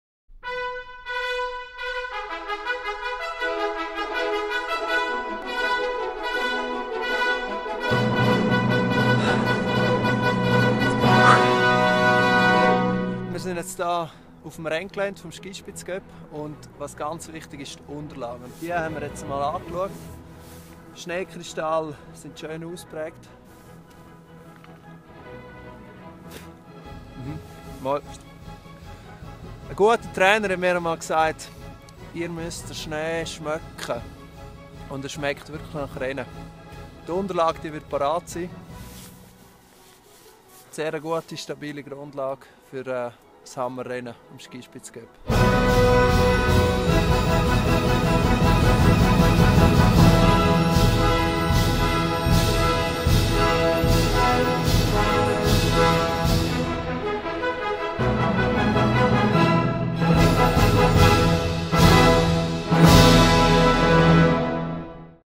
SkiSpitz - Schneeverhältnisse - Selfranga
Knapp zwei Wochen vor dem SkiSpitz-Cup melden wir "Pulver gut" am Selfrangalift!